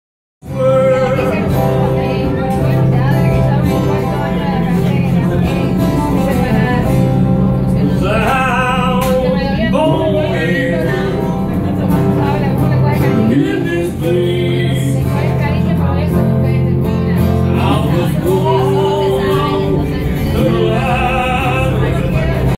Troy Youngblood and Dennis Stadelman performing on The TECO Line Streetcar!
Troy and Dennis perform for Streetcar Live Powered by Gasparilla Music Festival on January 2, 2016.